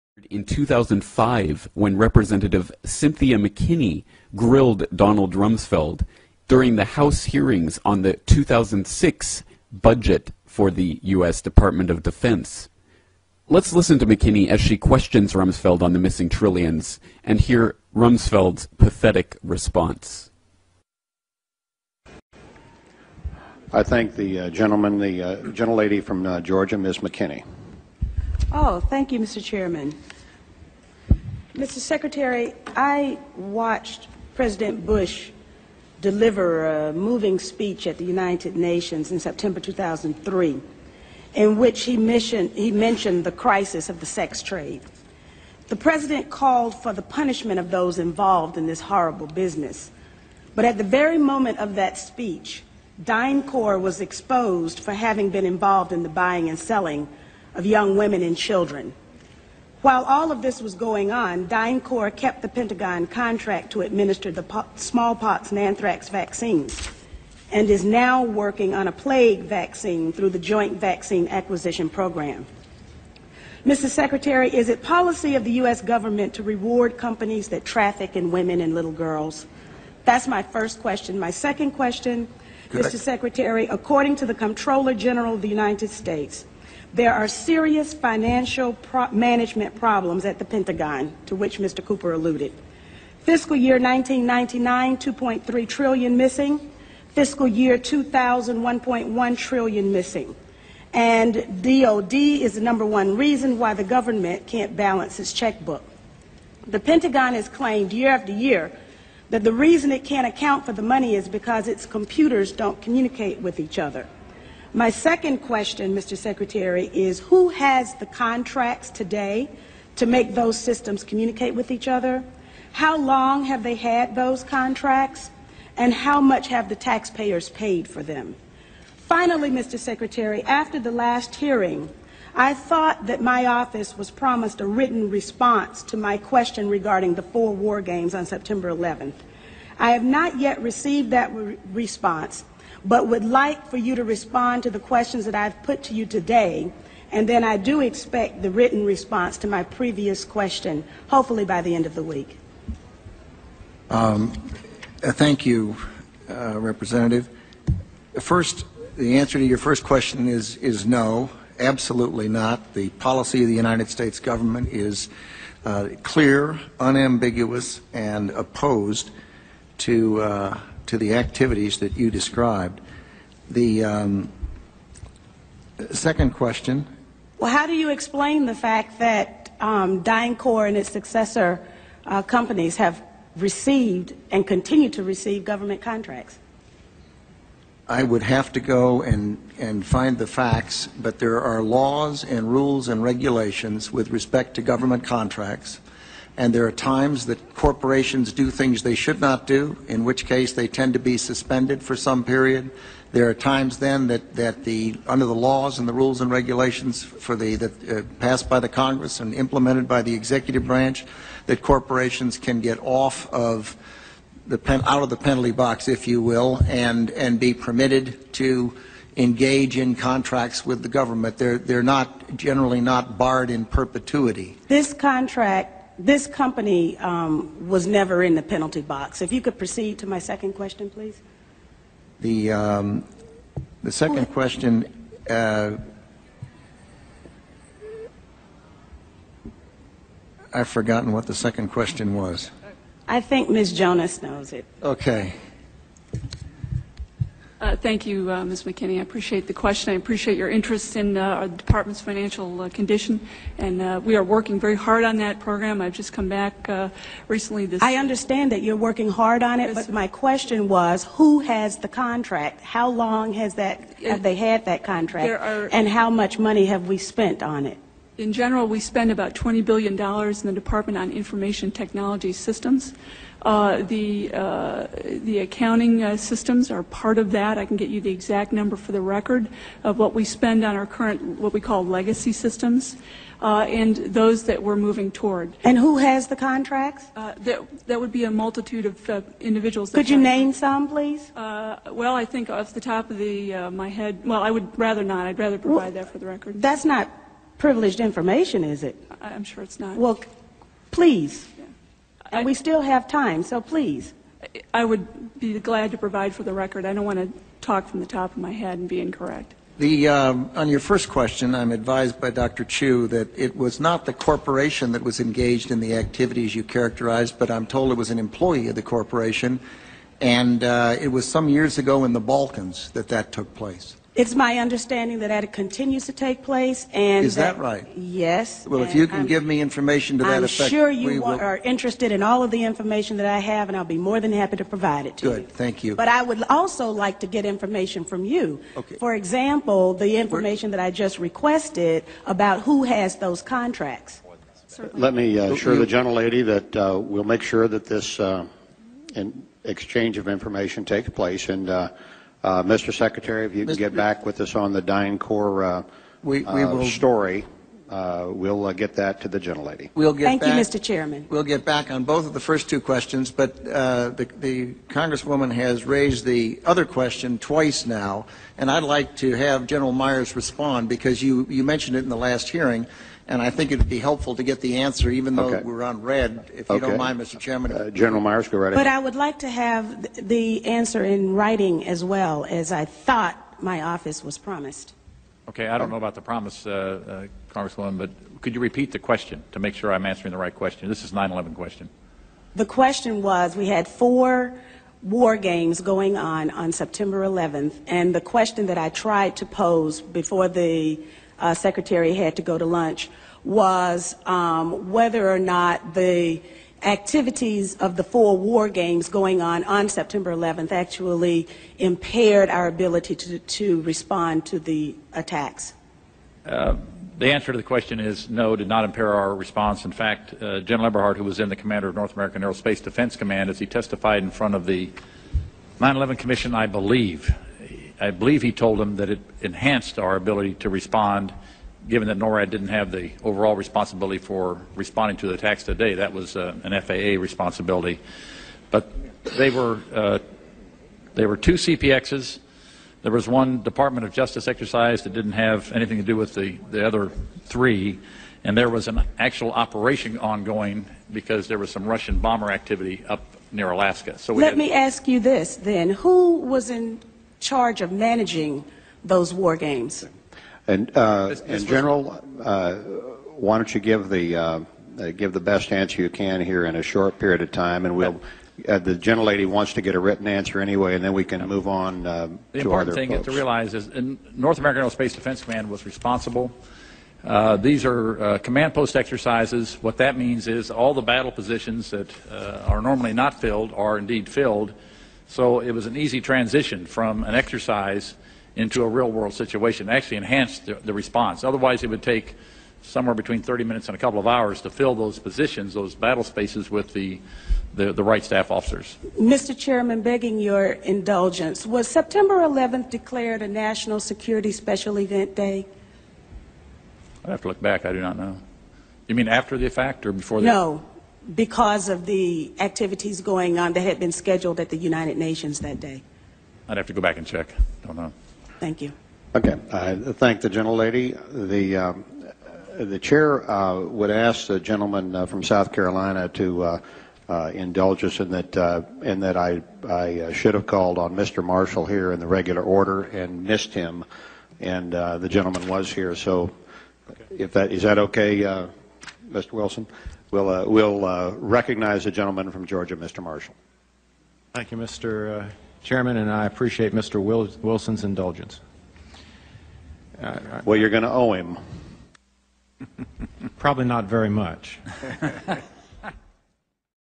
Cynthia McKinney Grills Donald Rumsfeld 360p
Cynthia Ann McKinney is Georgia's first African-American Congresswoman and an internationally known advocate for voting rights, human rights, and the strengthening of business ties between Africa and the United States.
CYNTHIA MCKINNEY GRILLS DONALD RUMSFELD WITH HARD QUESTIONS